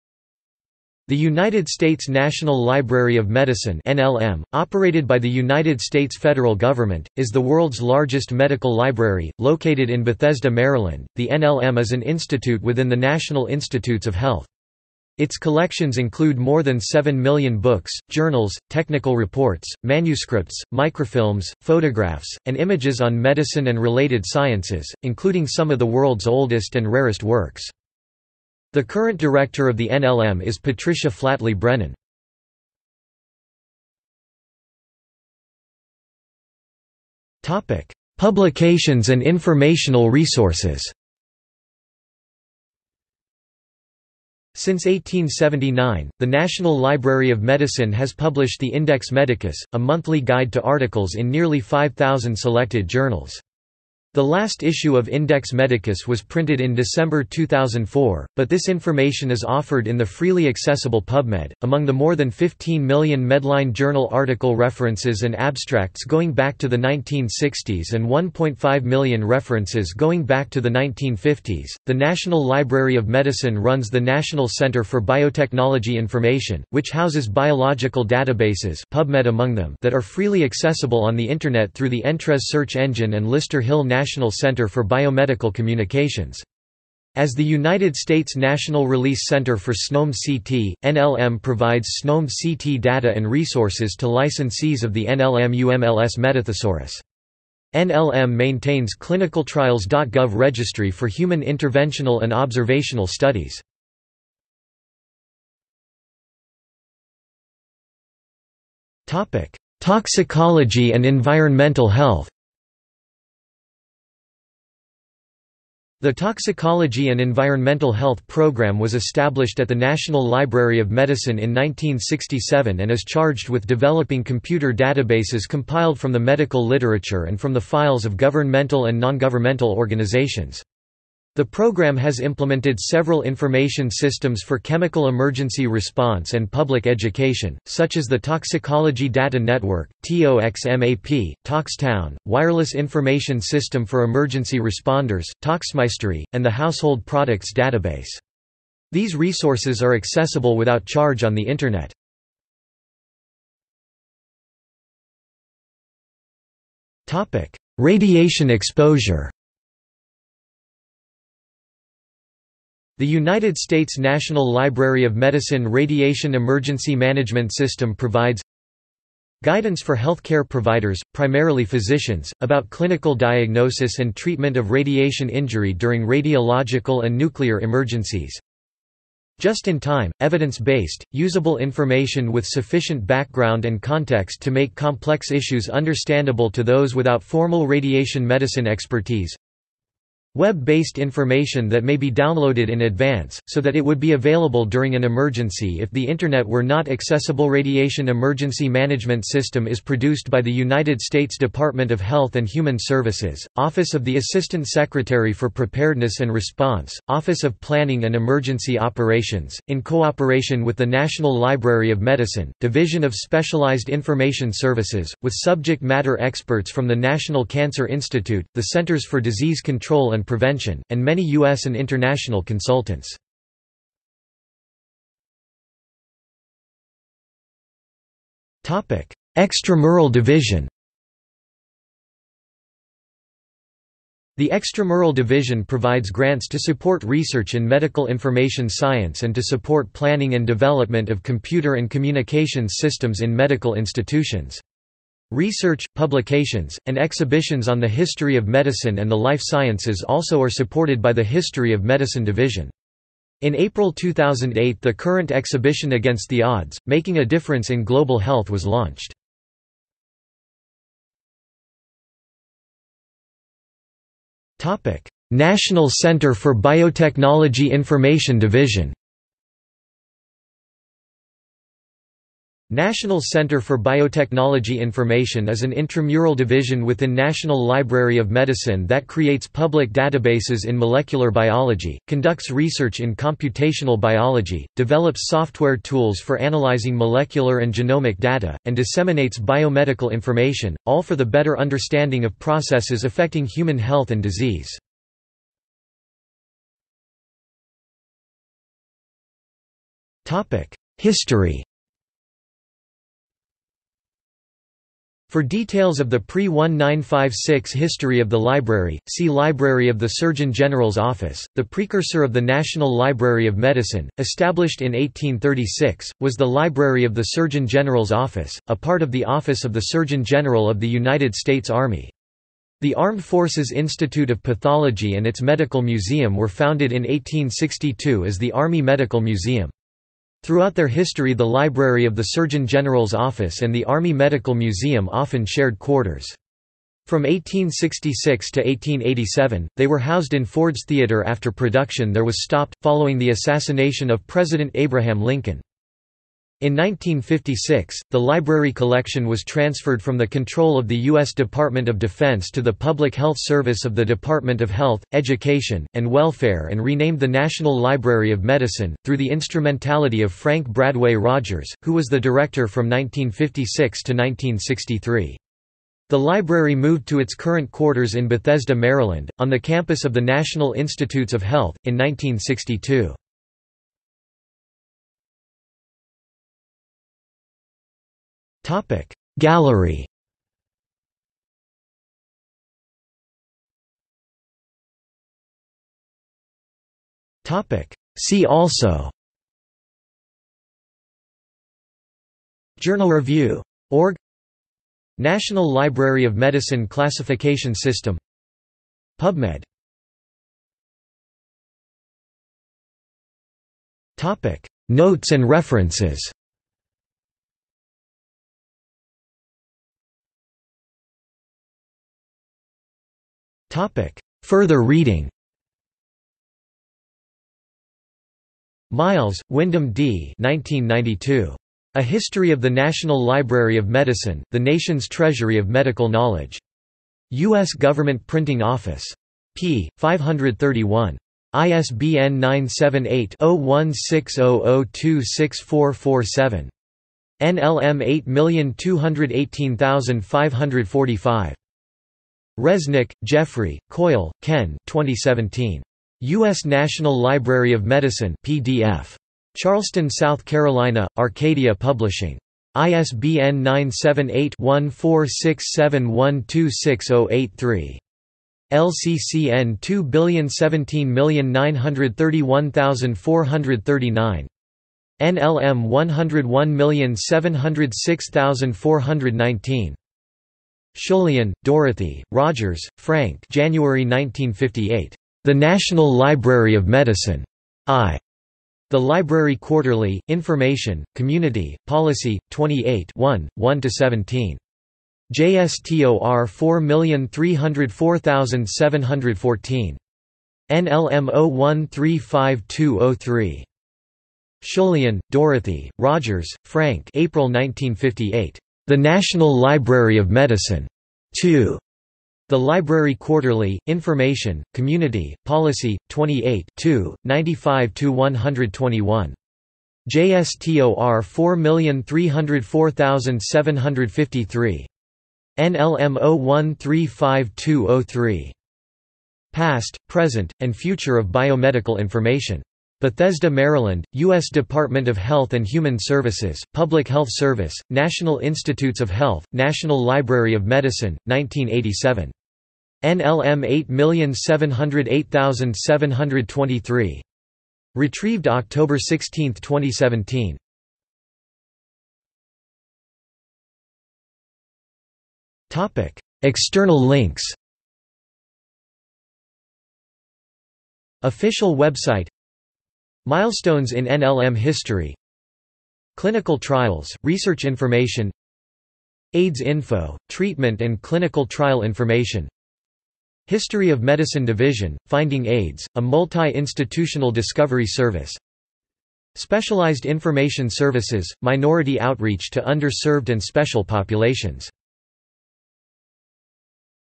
National Library of Medicine | Wikipedia audio article
00:00:33 1 Publications and informational resources
00:01:45 2 Toxicology and environmental health
00:02:33 3 Radiation exposure
00:03:45 4 Extramural division
00:04:21 5 National Center for Biotechnology Information division
00:04:56 6 History
00:06:15 7 Gallery
00:06:23 8 See also
00:06:40 9 Notes and references
00:06:50 10 Further reading
00:09:02 11 External links

- Socrates

SUMMARY
The United States National Library of Medicine (NLM), operated by the United States federal government, is the world's largest medical library.Located in Bethesda, Maryland, the NLM is an institute within the National Institutes of Health. Its collections include more than seven million books, journals, technical reports, manuscripts, microfilms, photographs, and images on medicine and related sciences, including some of the world's oldest and rarest works.
The current director of the NLM is Patricia Flatley Brennan.